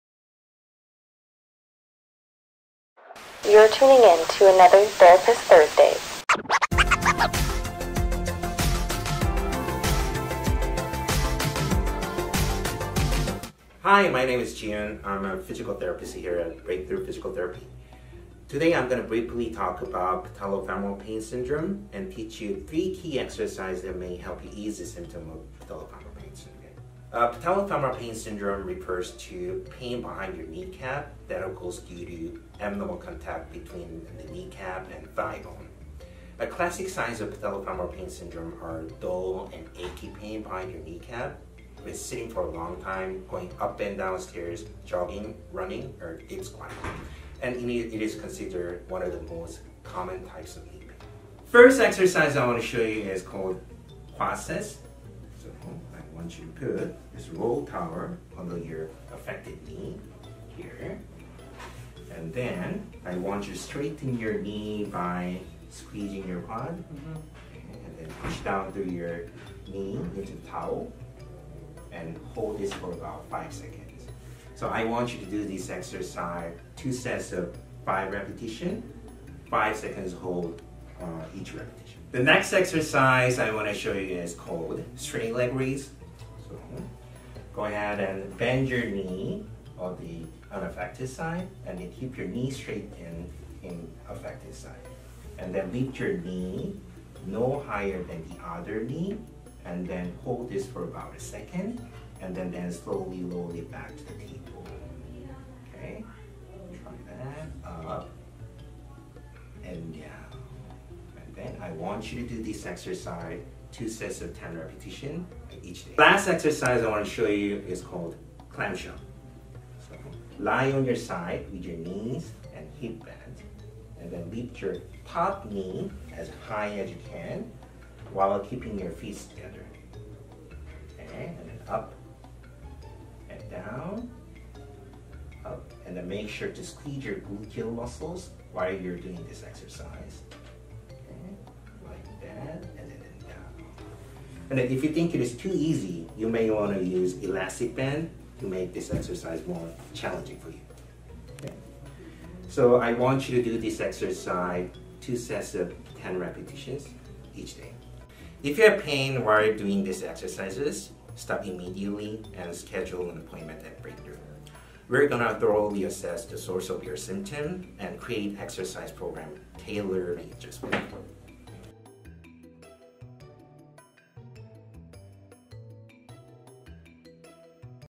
Patellofemoral Pain Syndrome
Patellofemoral pain syndrome is a condition in which the cartilage under the kneecap is damaged due to injury or overuse. It's common in people who participate in sports, especially women and young adults.
The most common symptom is knee pain that increases with stair climbing or squatting movements.